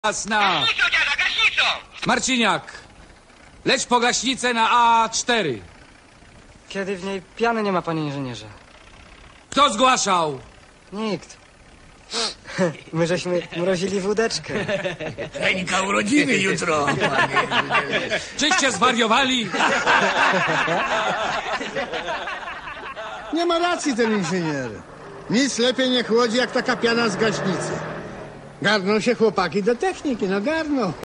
Chłodzenie wódeczki gaśnicą
-Marciniak, leć po gaśnicę na A4
-Kiedy w niej piany nie ma panie inżynierze
-Kto zgłaszał?
-Nikt... My żeśmy mrozili wódeczkę
-Heńka urodziny jutro
-Czyście zwariowali?
-Nie ma racji ten inżynier. Nic lepiej nie chłodzi jak taka piana z gaśnicy. Garną się chłopaki do techniki no garną.